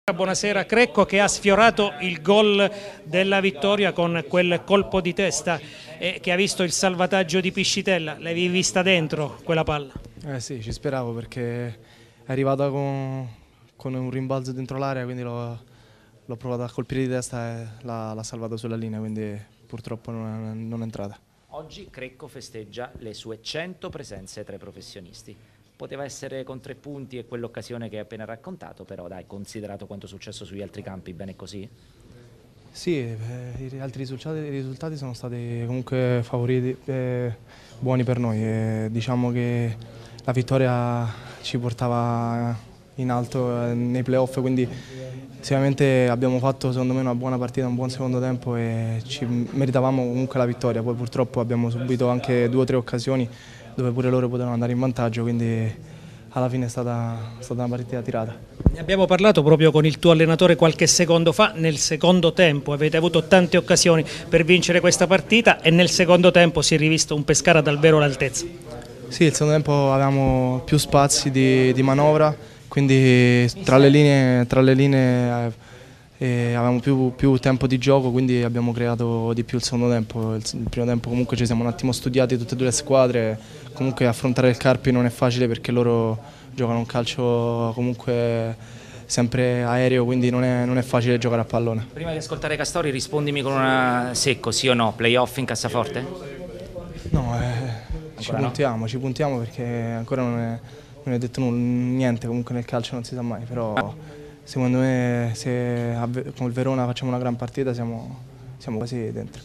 Carpi - Pescara 0-0: Luca Crecco
Intervista post gara al centrocampista del Pescara